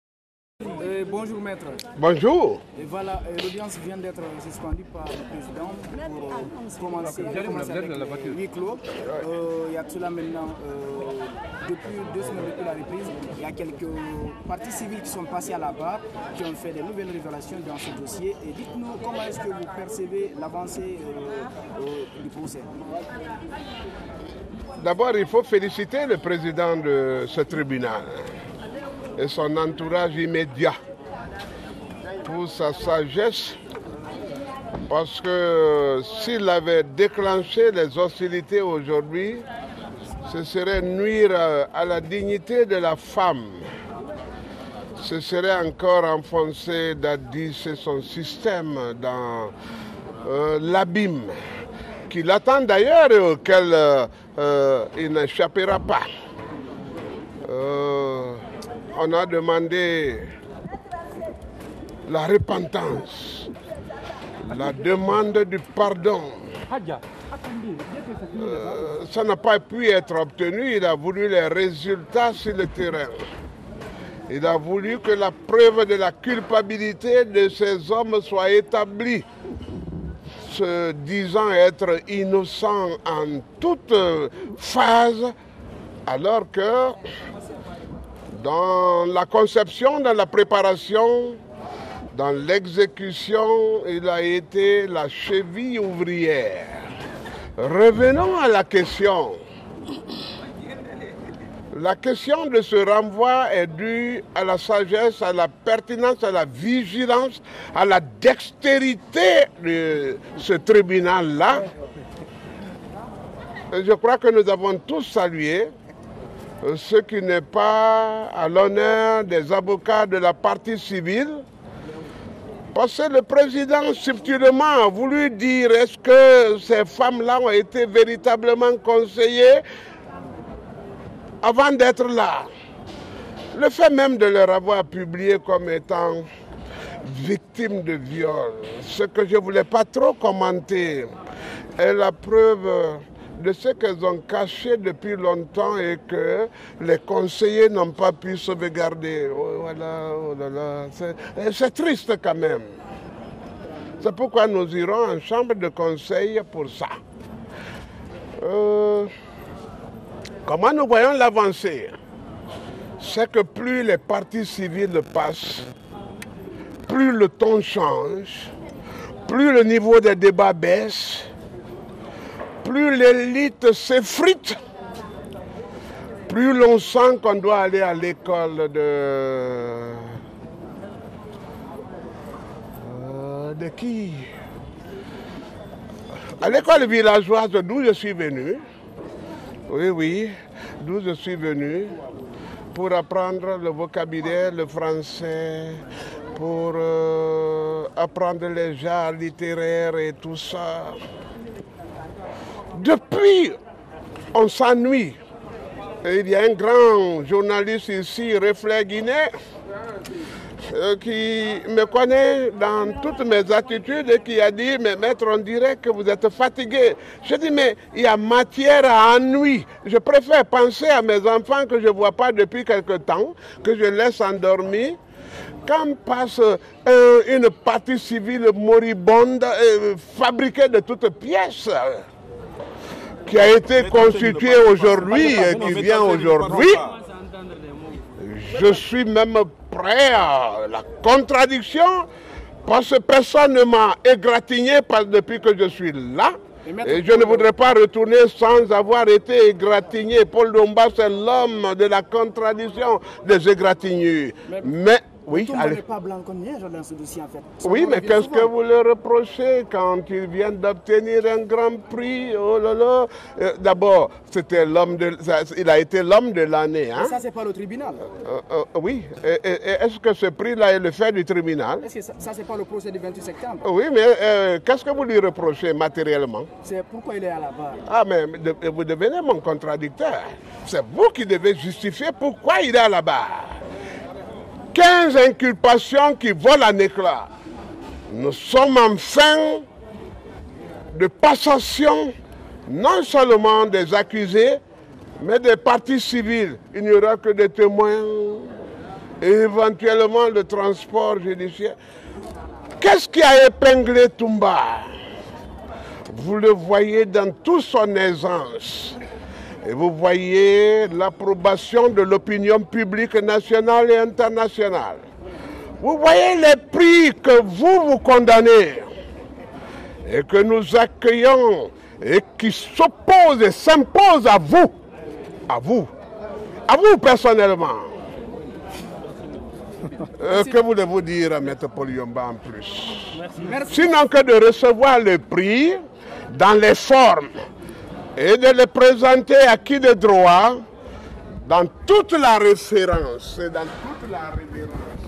Audition de victimes de viol au procès du 28 septembre: Dadis n’échappera pas à l’abîme» (Paul Yomba
Le procès des crimes perpétrés le 28 septembre 2009 s’est poursuivi ce mardi, 24 octobre 2023 au tribunal criminel de Dixinn délocalisé à la Cour d’appel de Conakry. Après la déposition de victimes et de parents de victimes, qui ont raconté l’horreur vécu au stade, le président du tribunal Ibrahima Sory 2 Tounkara, a suspendu l’audience. La démarche visait à faire sortir le public en vue de la déposition de victimes de viol à huis clos. Maître Paul Yomba Kourouma, un des avocats de Toumba Diakité, ancien aide de camp du chef de la junte de 2009, a mis l’occasion à profit pour tirer à boulets rouges sur Moussa Dadis Camara, a constaté sur place Guineematin.com à travers un de ses reporters.

 Après avoir entendu trois parties civiles, le juge a ordonné de libérer la salle pour permettre au tribunal d’entendre deux femmes, victimes de viol le lundi 28 septembre 2009.
Maître Paul Yomba Kourouma, un des avocats de Toumba Diakité, a salué la démarche du président du tribunal avant de promettre l’échec de Moussa Dadis Camara dans ce procès. « D’abord, il faut féliciter le président de ce tribunal parce que s’il avait déclenché les hostilités aujourd’hui, ce serait nuire à la dignité de la femme, ce serait encore enfoncer Dadis et son système dans l’abîme qui l’attend d’ailleurs et auquel il n’échappera pas. On a demandé la repentance, la demande du pardon ; ça n’a pas pu être obtenu. Il a voulu le résultat sur le terrain, il a voulu que la preuve de la culpabilité de ses hommes soit établie, se disant être innocent en toutes phases, alors que dans la conception, la préparation, dans l’exécution, il a été la cheville ouvrière. La question de ce renvoi est due à la sagesse, à la pertinence, à la vigilance, à la dextérité de ce tribunal. Et je crois que nous avons tous salué ce qui n’est pas à l’honneur des avocats de la partie civile », a dit l’avocat, dans un entretien après la suspension de l’audience.